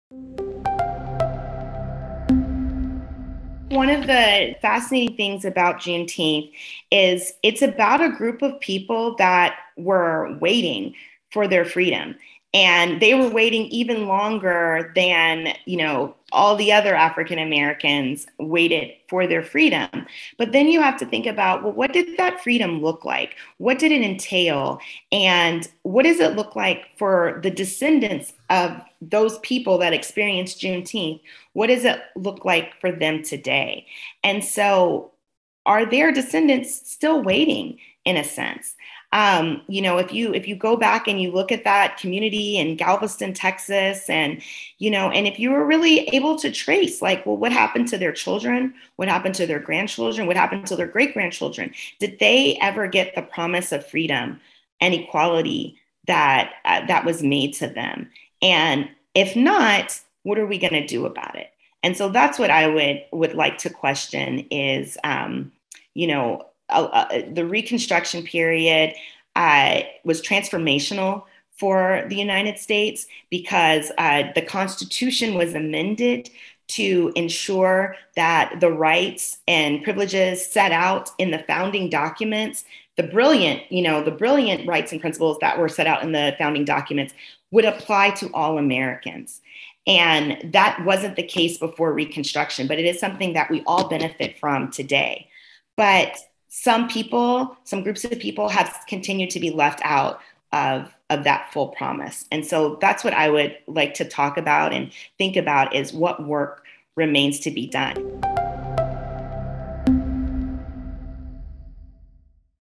After Juneteenth: The Promise of Freedom | Michelle Coles
America is finally reckoning with its racial past. It is commendable and heartening to see that Juneteenth has at last been recognized as a federal holiday. But there is still so much work to be done. Black Americans can’t be kept on hold indefinitely, like the enslaved people in Galveston who waited years for the message of freedom to reach them.

Michelle Coles, a heroic civil rights lawyer and award-winning YA author, illuminates what the promise of freedom meant to enslaved Black people, what Juneteenth means today, and how we can learn from history by honestly assessing the legacy of segregation and disenfranchisement.